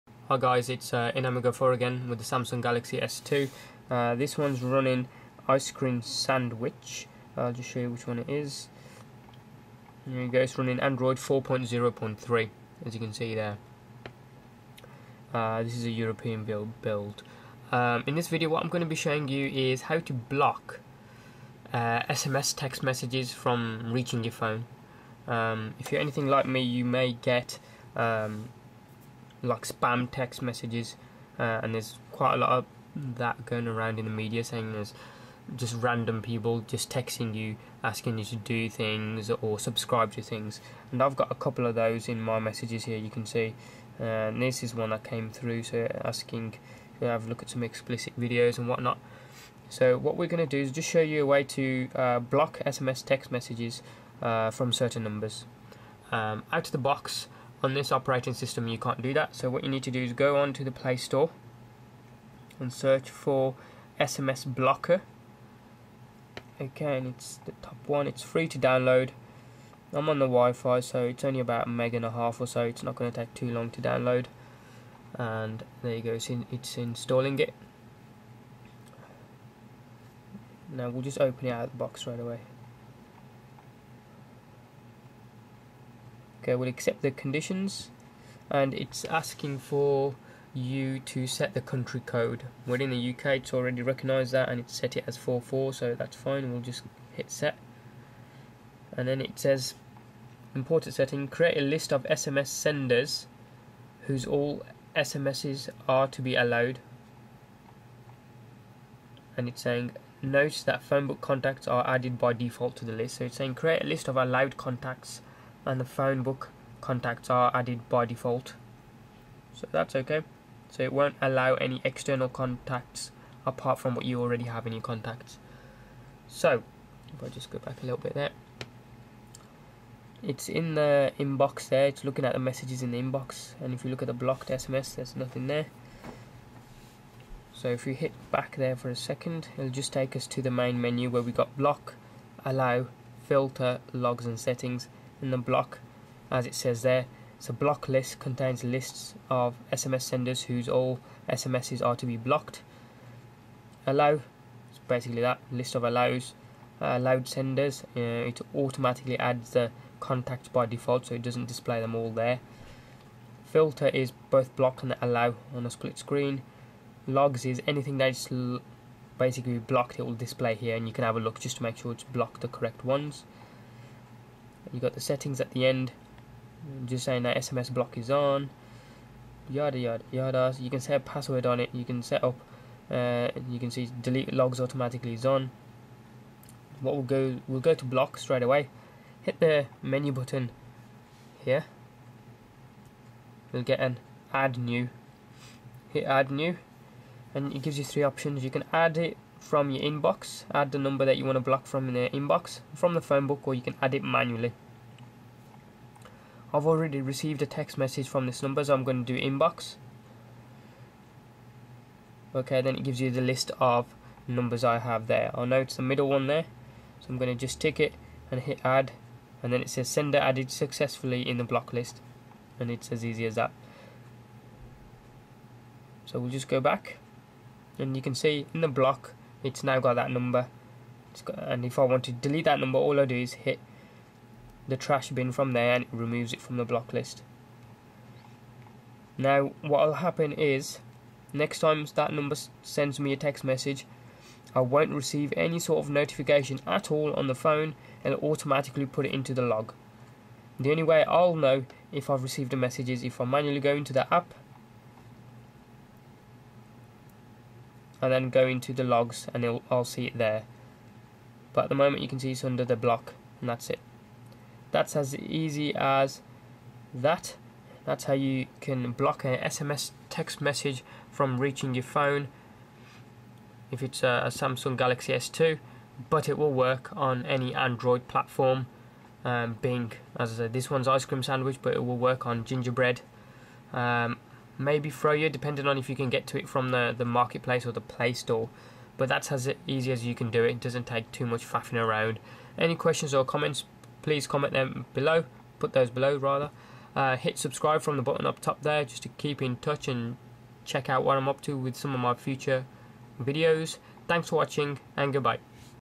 How to Block Nuisance / Spam SMS Text Messages, Android
Quickly show you how to block Nuisance / Spam sms text messages on a Android device, I am showing you on my Samsung Galaxy S2 / SII which is running Ice Cream Sandwich (ICS) 4.0.3